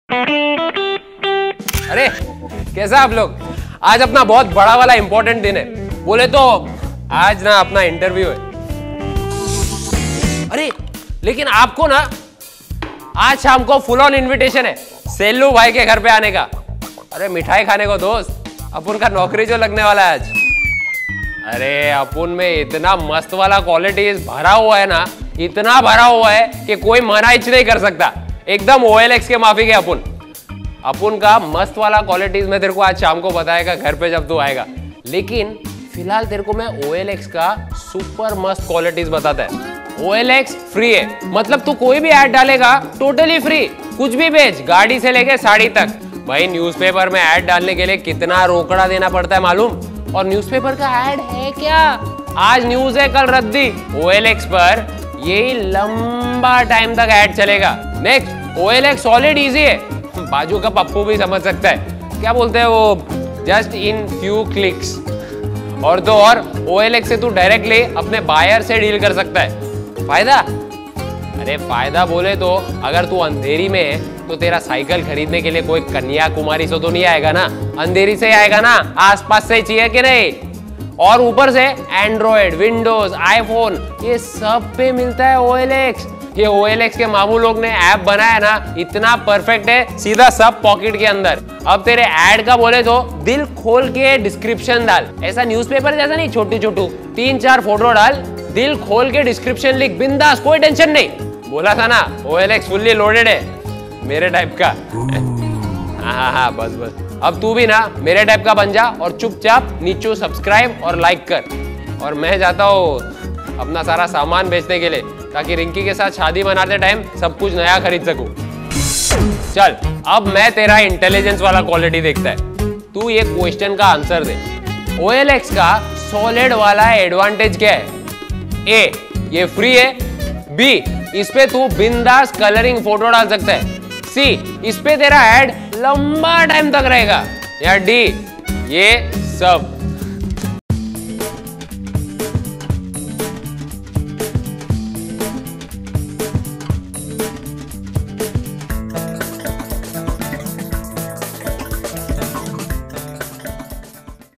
Advantages of OLX
What is the ‘faayda’ of posting the ad of your “used-less” stuff on OLX? Sellu Bhai talks about the “super-mast” qualities of OLX.

Free classifieds. India's Largest Marketplace for Used Goods.